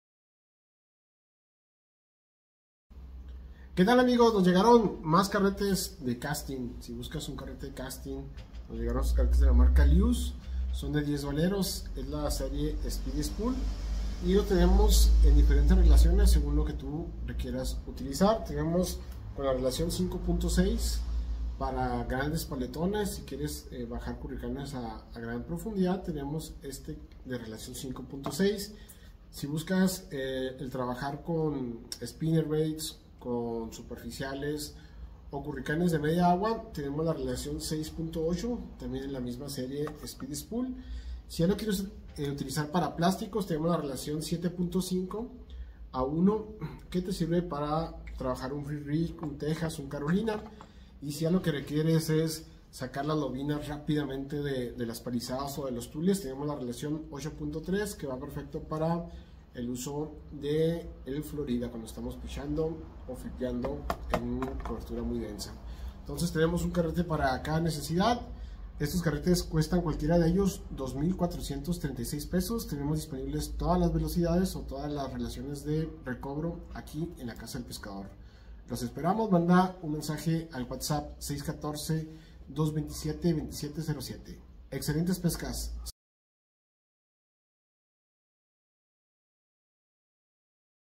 ¿Cuál carrete de casting elegir?
Que relación de recobro es la adecuada para cada técnica!